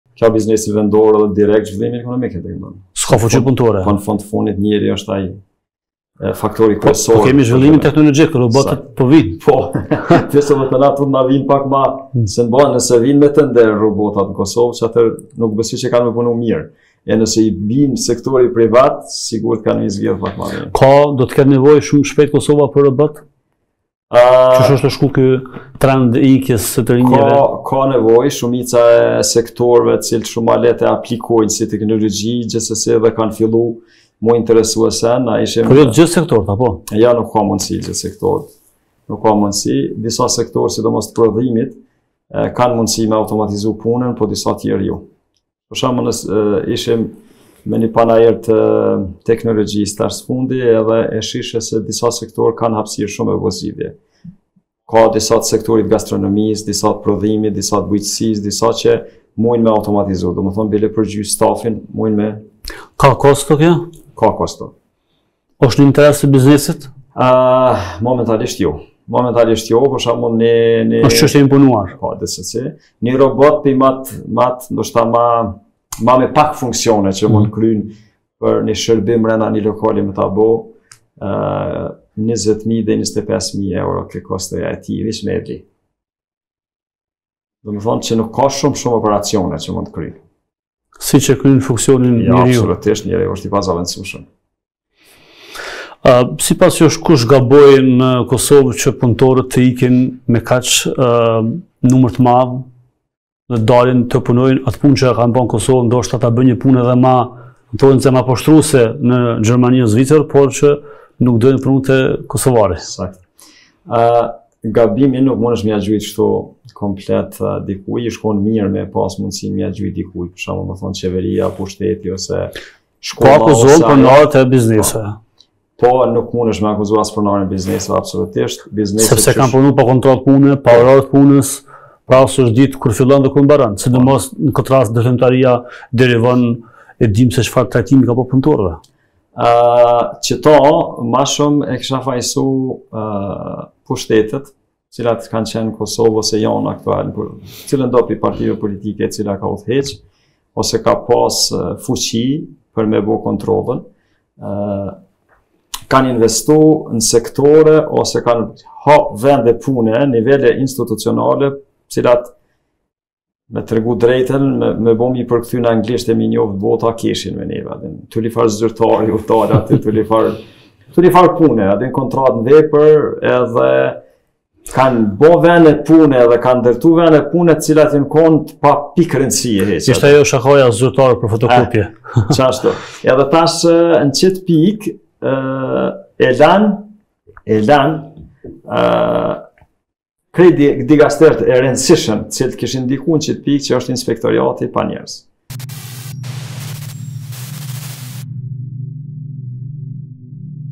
Mungesa e fuqisë punëtore po e dëmton biznesin privat. Flet ekonomisti, Fatmir Smajli - RTI
Të gjitha të drejtat e rezervuara për televizionin RTI.